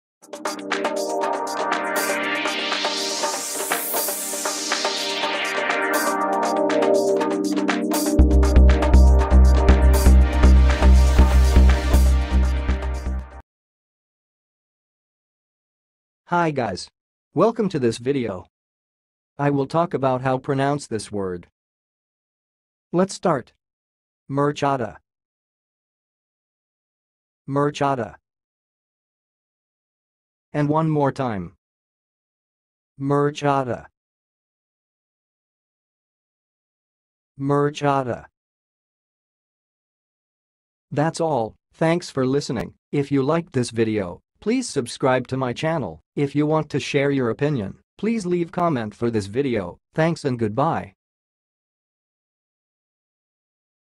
MURCHADA - HOW TO PRONOUNCE IT!?
Watch how to say and pronounce "murchada"!